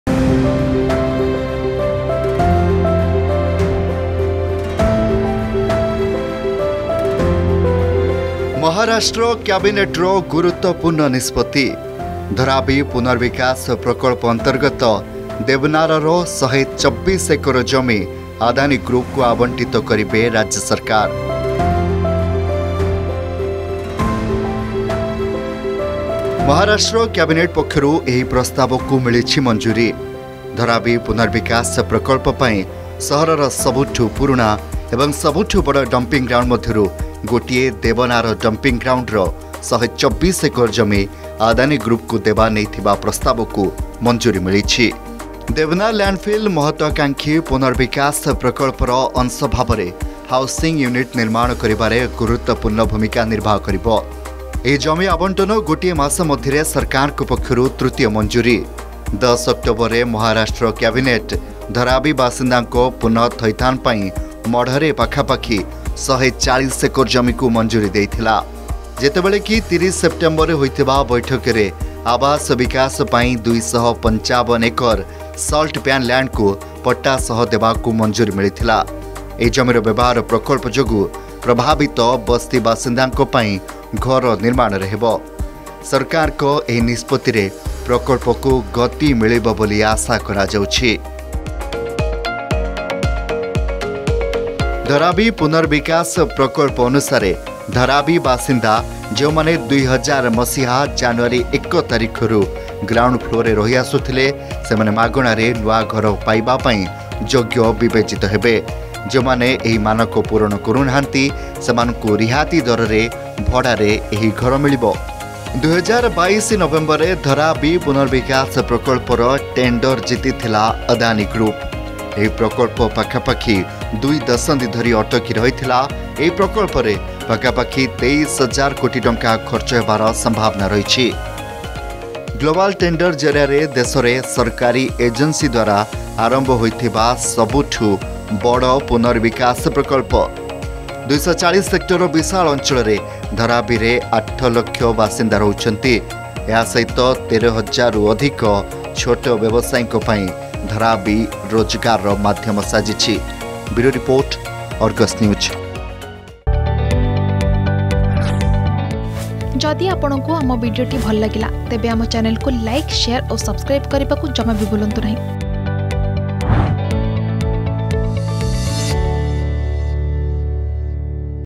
ଦେବନାରର ୧୨୪ ଏକର ଜମି ଆଦାନୀ ଗ୍ରୁପକୁ ଆବଣ୍ଟିତ କରାଯିବ | Adani Group | Argus News
ମହାରାଷ୍ଟ୍ର କ୍ୟାବିନେଟର ଗୁରୁତ୍ବପୂର୍ଣ୍ଣ ନିଷ୍ପତ୍ତି ।  ଧରାବୀ ପୁନର୍ବିକାଶ ପ୍ରକଳ୍ପ ଅନ୍ତର୍ଗତ ଦେବନାରର ୧୨୪ ଏକର ଜମି ଆଦାନୀ ଗ୍ରୁପକୁ ଆବଣ୍ଟିତ କରିବେ ରାଜ୍ୟ ସରକାର। ଏହି ଜମିରେ ପ୍ରଭାବିତ ବସ୍ତି ବାସିନ୍ଦାଙ୍କ ପାଇଁ ନିର୍ମାଣ ହେବ ଘର  ।